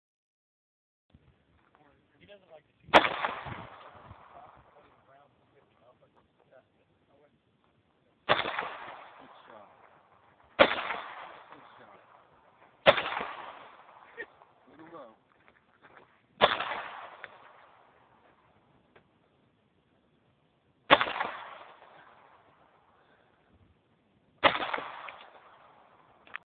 Seth age 10 shooting Michael Crow's Remington Model 1911 on 12/02/2012
10 old year Seth Shooting Michael Crow's Remington Model 1911 on 12/2/2012